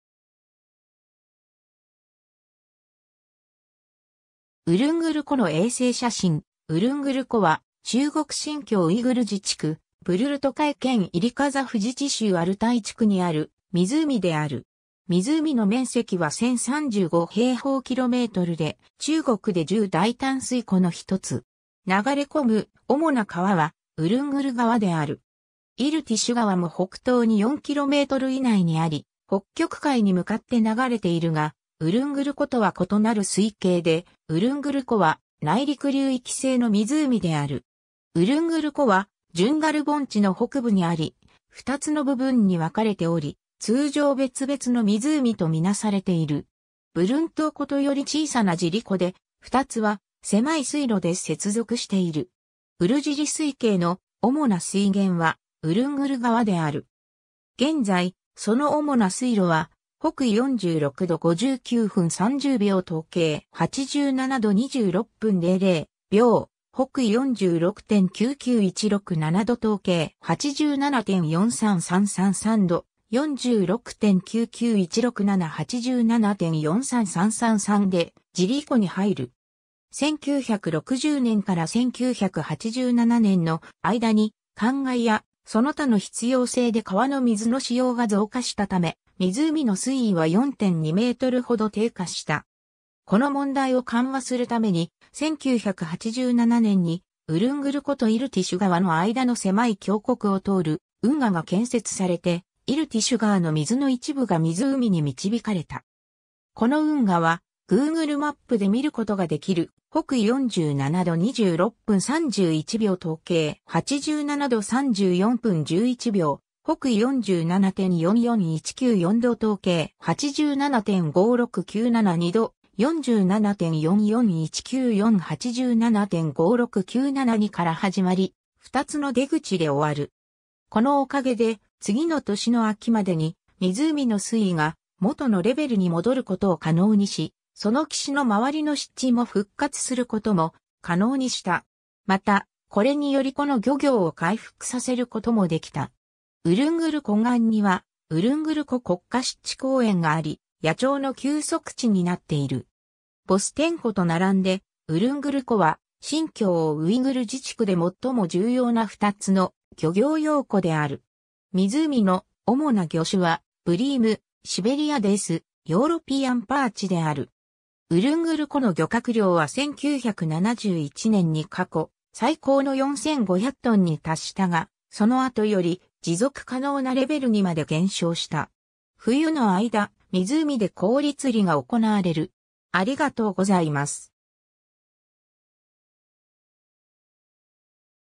ウルングル湖
ウルングル湖の衛星写真 ウルングル湖（ウルングルこ、中国語: 乌伦古湖、英語: Ulungur Lake）は中国新疆ウイグル自治区ブルルトカイ県イリ・カザフ自治州アルタイ地区にある湖である。
湖の面積は1,035平方キロメートルで、中国で十大淡水湖のひとつ。
流れ込む主な川はウルングル川である。
イルティシュ川も北東に4km以内にあり、北極海に向かって流れているが、ウルングル湖とは異なる水系で、ウルングル湖は内陸流域性の湖である。
 ウルングル湖はジュンガル盆地の北部にあり、2つの部分に分かれており、通常別々の湖と見なされている。
ブルントゥオ湖（つまりウルングル湖）とより小さなジリ湖（吉力湖、 北緯46度55分00秒 東経87度25分00秒 / 北緯46.91667度 東経87.41667度 / 46.91667; 87.41667）で、2つは狭い水路で接続している。
ウルングル＝ジリ水系の主な水源はウルングル川である。
現在、その主な水路は 北緯46度59分30秒 東経87度26分00秒 / 北緯46.99167度 東経87.43333度 / 46.99167; 87.43333でジリ湖に入る。
 1960年から1987年の間に、灌漑やその他の必要性で川の水の使用が増加したため、湖の水位は4.2mほど低下した。
この問題を緩和するために、1987年にウルングル湖とイルティシュ川の間の狭い峡谷を通る運河が建設されて、イルティシュ川の水の一部が湖に導かれた。
この運河はGoogle マップで見ることができる： 北緯47度26分31秒 東経87度34分11秒 / 北緯47.44194度 東経87.56972度 / 47.44194; 87.56972から始まり、2つの出口（ 北緯47度25分16.3秒 東経87度33分04.6秒 / 北緯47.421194度 東経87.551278度 / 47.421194; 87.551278と 北緯47度25分21.6秒 東経87度33分27.5秒 / 北緯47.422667度 東経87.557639度 / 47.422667; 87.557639）で終る。
このおかげで、次の年の秋までに湖の水位が元のレベルに戻ることを可能にし、その岸の周りの湿地も復活することも可能にした。
また、これにより湖の漁業を回復させることもできた。
 ウルングル湖岸には「ウルングル湖国家湿地公園」があり、野鳥の休息地になっている。
 ボステン湖と並んで、ウルングル湖は新疆ウイグル自治区で最も重要な2つの漁業用湖である。
湖の主な魚種はブリーム、シベリア・デイス（Siberian dace）、ヨーロピアンパーチである。
ウルングル湖の漁獲量は1971年に過去最高の4,500トンに達したが、その後より持続可能なレベルにまで減少した。
 冬の間、湖で氷釣りが行われる。